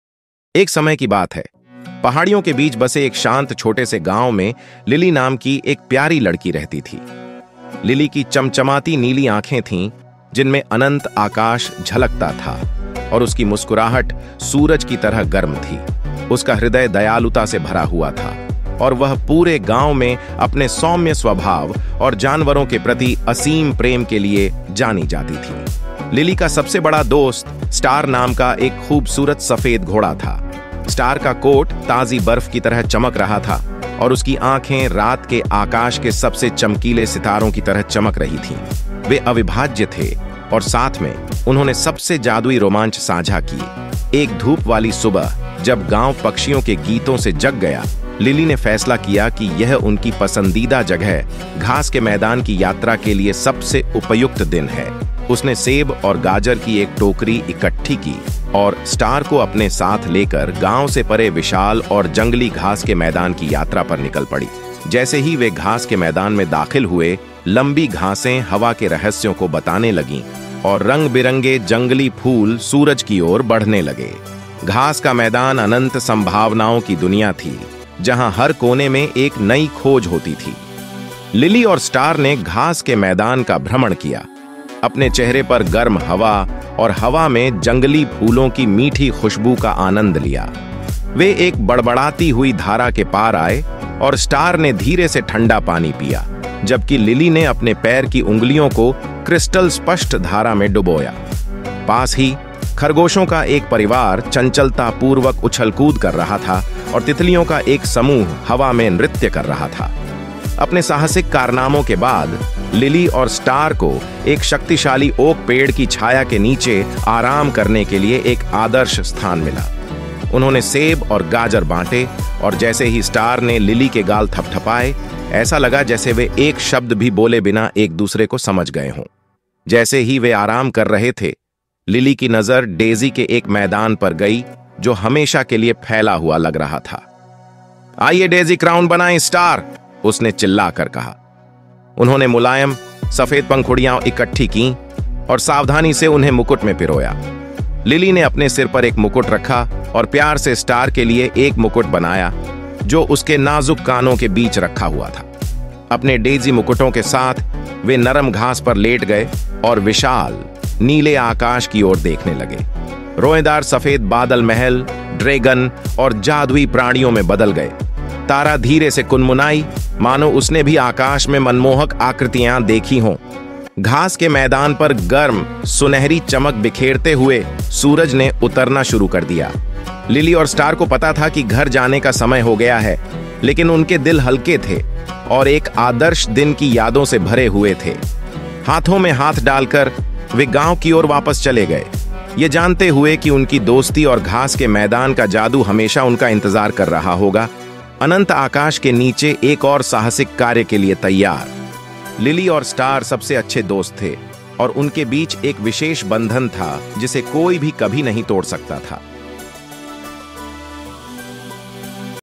Lily-& Star The Best Friend I Cute Little Gril Story I Hindi Story For Kids
🌼 Lily-& Star The Best Friend

Once upon a time, in a peaceful little village nestled between rolling hills, lived a lovely girl named Lily. Lily had sparkling blue eyes that reflected the endless sky and a smile as warm as the sun. Her heart was filled with kindness, and she was known throughout the village for her gentle nature and boundless love for animals.

Lily's greatest friend was a beautiful white horse named Star. Star's coat shimmered like fresh snow, and her eyes sparkled like the brightest stars in the night sky. They were inseparable, and together, they shared the most magical adventures.

One sunny morning, as the village awoke to the songs of birds, Lily decided it was the perfect day to visit their favorite place, the meadow. She gathered a basket of apples and carrots and, with Star by her side, set off on a journey to the vast and wild meadow that lay beyond the village.

As they entered the meadow, the tall grasses whispered secrets to the wind, and colorful wildflowers reached for the sun. The meadow was a world of endless possibilities, where every corner held a new discovery.

Lily and Star explored the meadow, enjoying the warm breeze on their faces and the sweet scent of wildflowers in the air. They came across a babbling brook, and Star gently sipped the cool water while Lily dipped her toes into the crystal-clear stream. Nearby, a family of rabbits hopped playfully, and a group of butterflies danced in the air.

After their adventures, Lily and Star found the perfect spot to rest under the shade of a mighty oak tree. They shared the apples and carrots, and as Star nuzzled Lily's cheek, it was as if they understood each other without speaking a word.

As they relaxed, Lily's eyes were drawn to a field of daisies that seemed to stretch on forever. "Let's make daisy crowns, Star!" she exclaimed. They gathered the soft, white petals and carefully wove them into crowns. Lily placed a crown on her own head and lovingly fashioned one for Star, which rested between her delicate ears.

With their daisy crowns, they lay back on the soft grass and gazed up at the vast, blue sky. The fluffy white clouds transformed into castles, dragons, and magical creatures. Star whinnied softly, as if she, too, saw the enchanting shapes in the sky.

The sun began its descent, casting a warm, golden glow over the meadow. Lily and Star knew it was time to head home, but their hearts were light and filled with the memories of a perfect day.

Hand in hoof, they made their way back to the village, knowing that their friendship and the magic of the meadow would always be waiting for them, ready for another adventure under the endless sky. Lily and Star were the best of friends, and they had a special bond that no one could ever break.